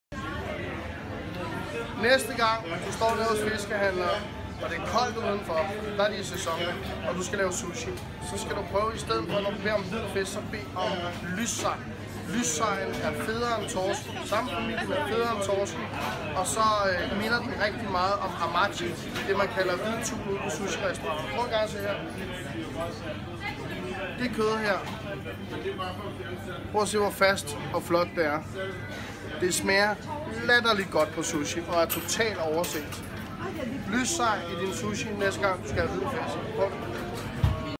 Dansk fisk: Lyssej til sushi
Lyssej er federe end torsk og minder om 'hamachi' - det man kalder hvid tun på sushirestauranter. Prøv den næste gang du skal lave sushi.